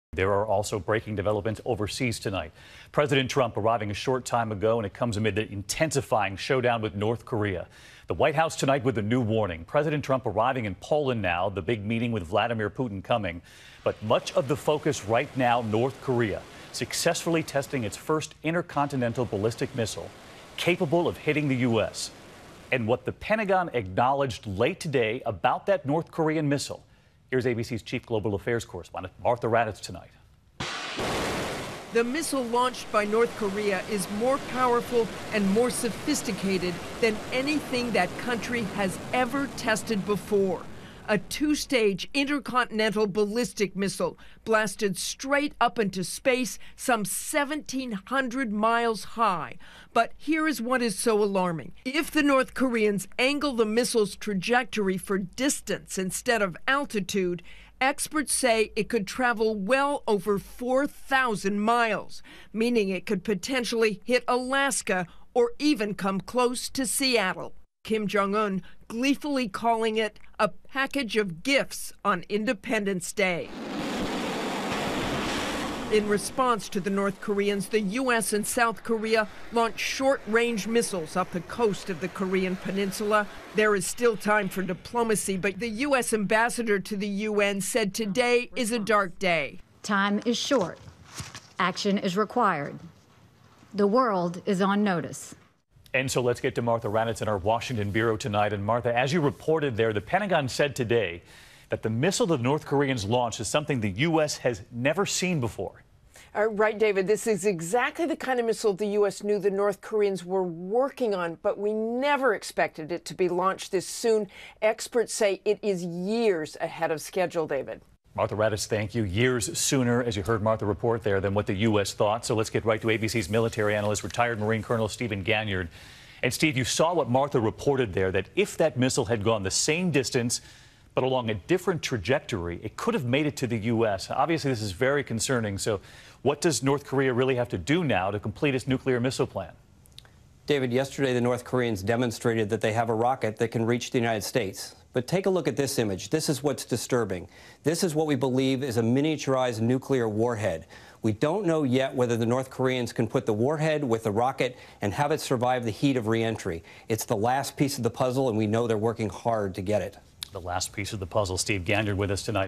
North Korea Successfully Tests A Ballistic Missile Capable Of Hitting The US
July 5, 2017 - Trump criticizes China for failing to put more pressure on North Korea and their nuclear program.